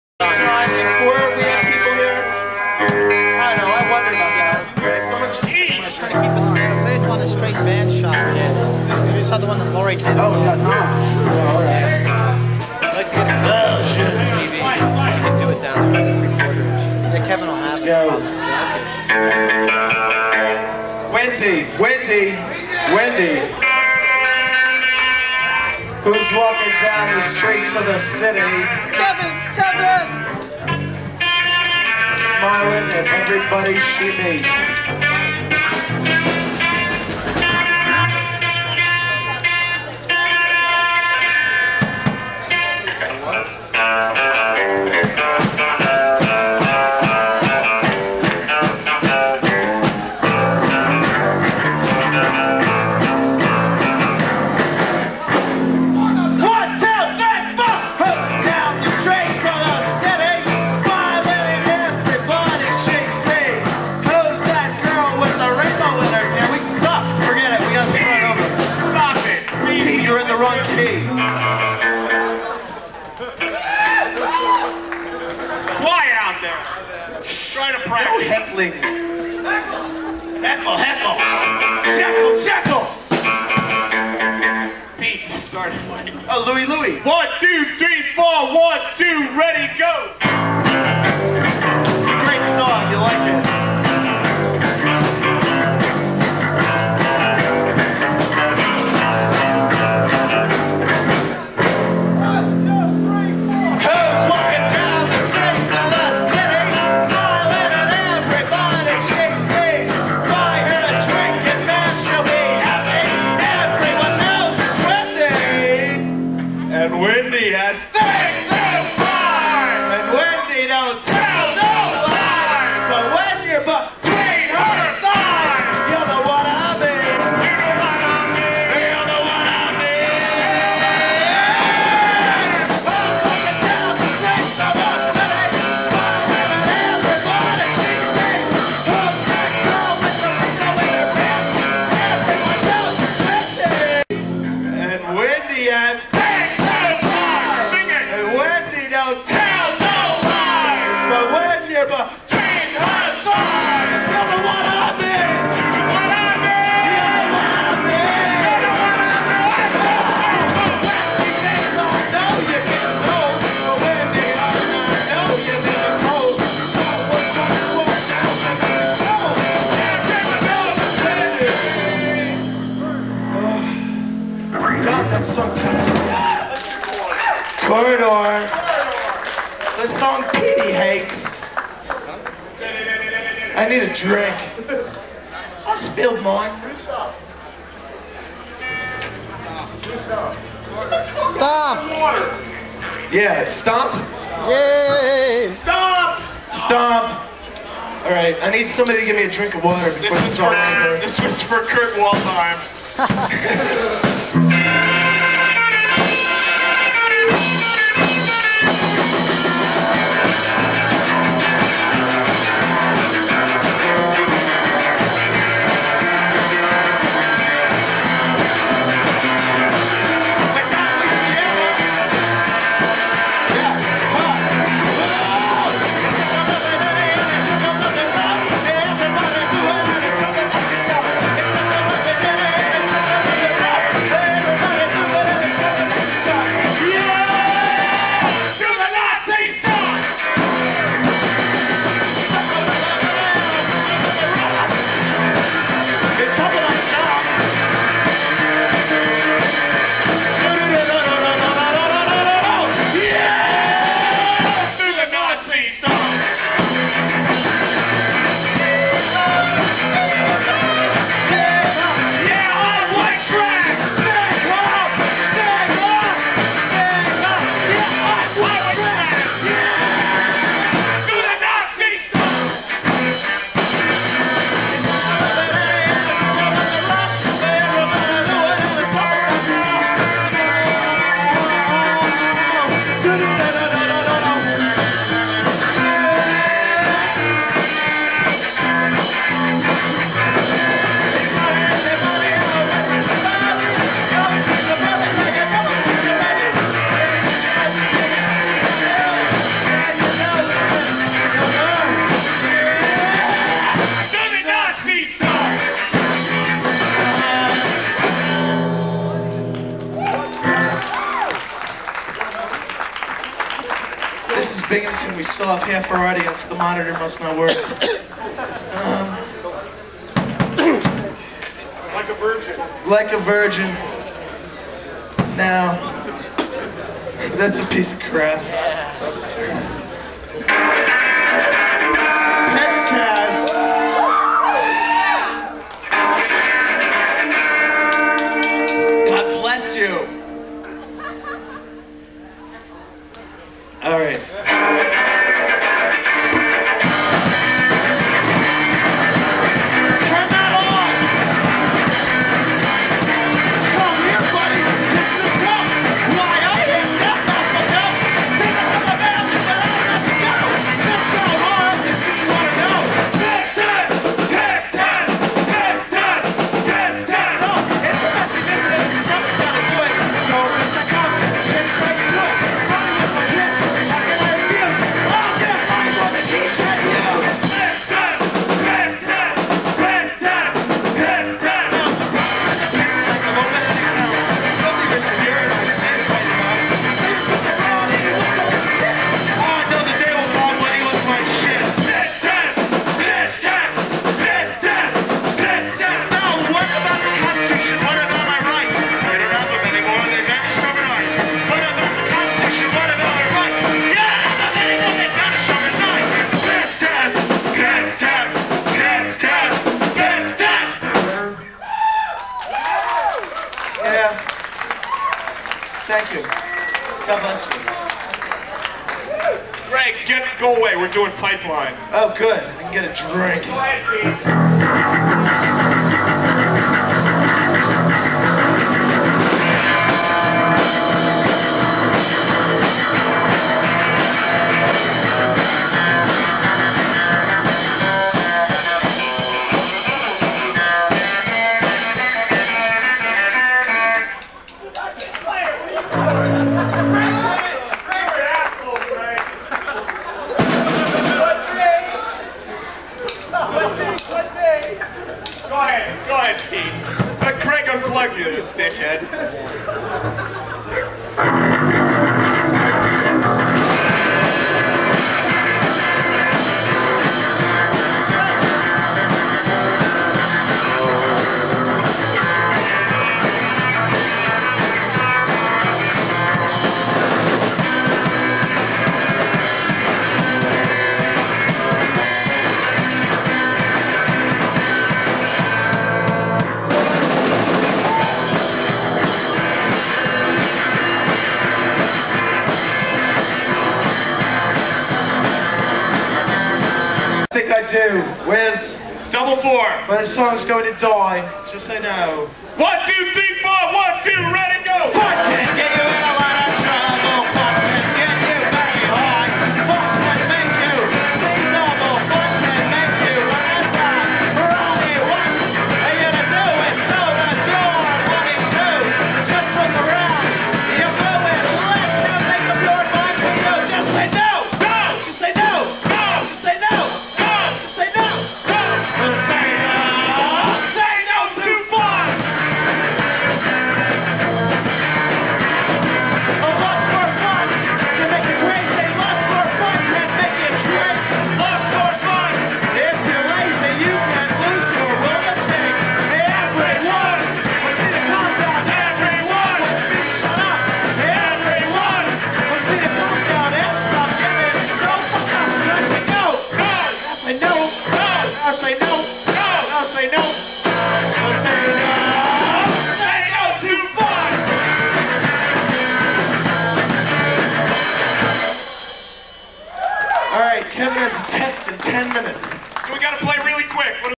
Iron Wombat 1986 Part 5
Founded in 1986 at Suny-Binghamton. Their motto was "any dick can do this". Their first gig was opening for The Dead Milkmen. The stipulation was that the Milkmen would get to name them and so it was that Iron Wombat was born. Not the most polished outfit they influenced a lot of other young people to pick up guitars and drums and get up onstage. Having fun and "taking the piss" was their goal. Enjoy.